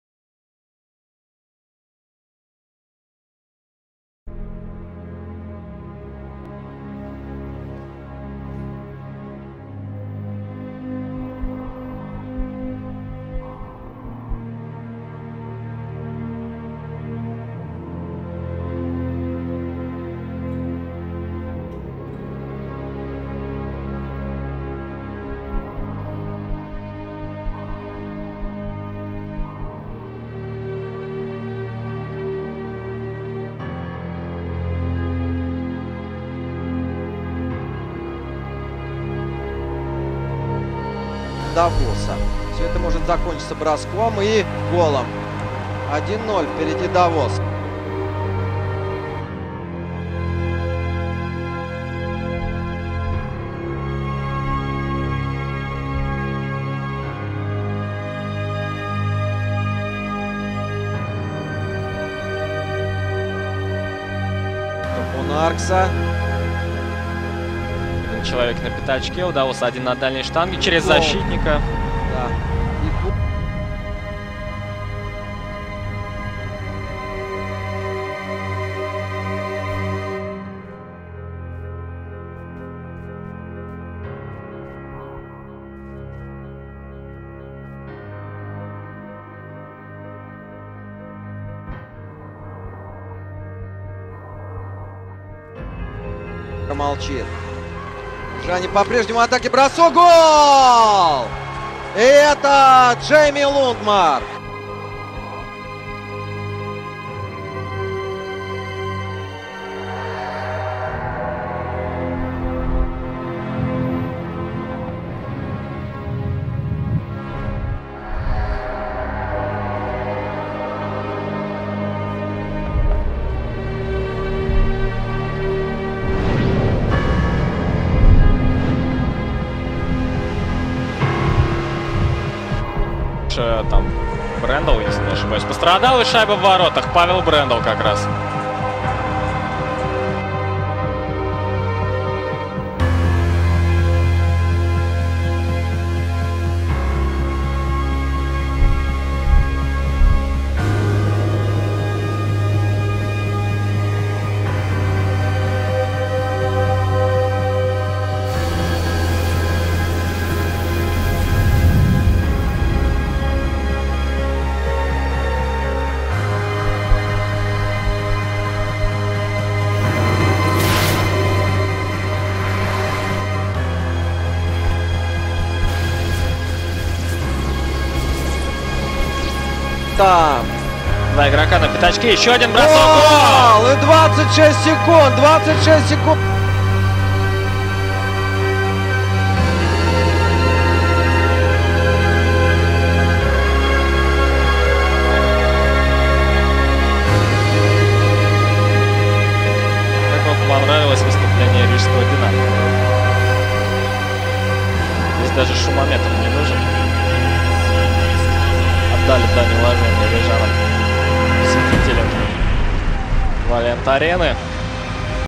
spengler cup 2011 final Dinamo Riga vs HC Davos 2-3
Dinamo Riga - HC Davos 2-3